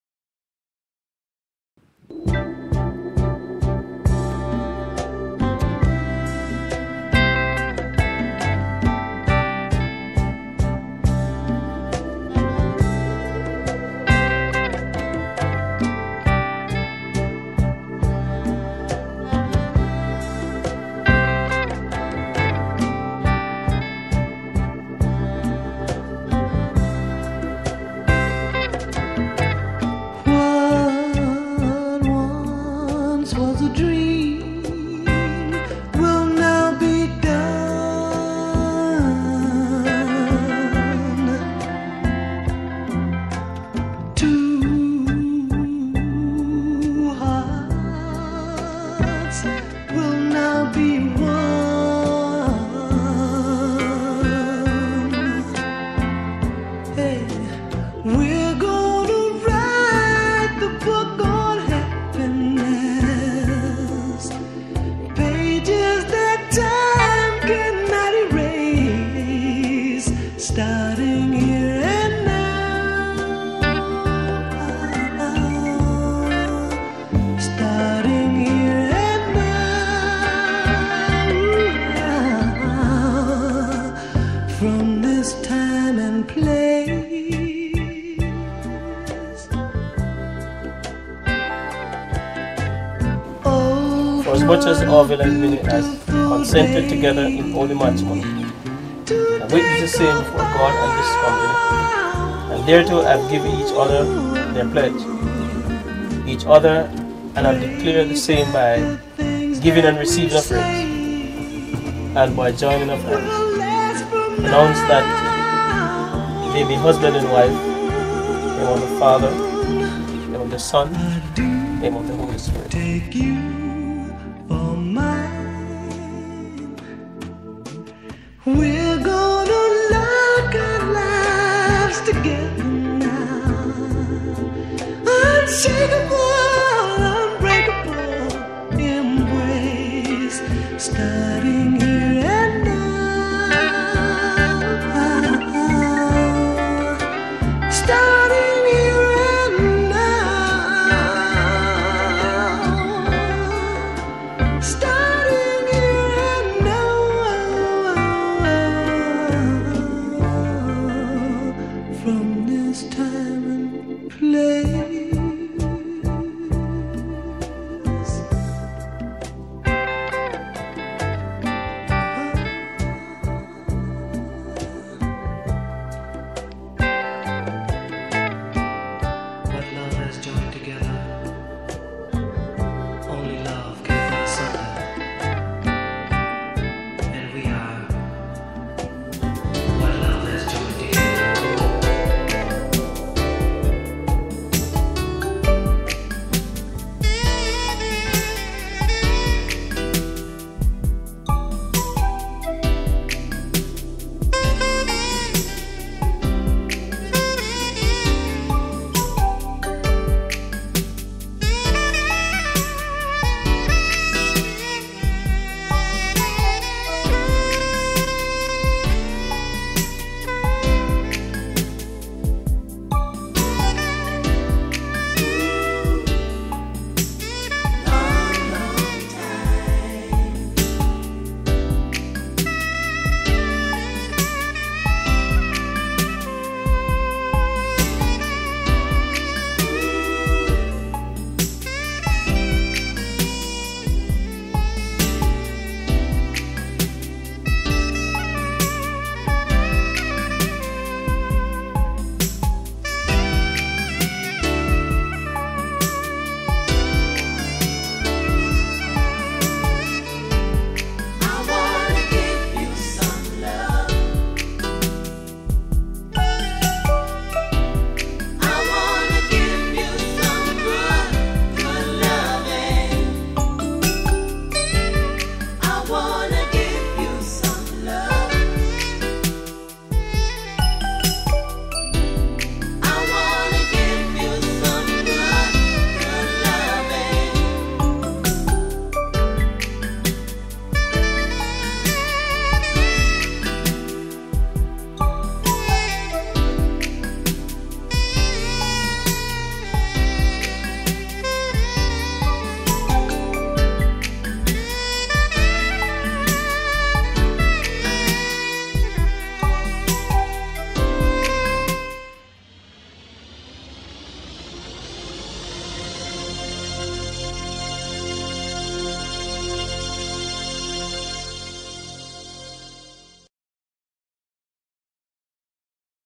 Jamaica Wedding Videographer
ORET & MINNIE'S WEDDING ON THE RIVER  in 2010. The couple tied the knot on board a boat cruising down the river at the Goldeneye Resort in the scenic parish of St Mary, eastern Jamaica.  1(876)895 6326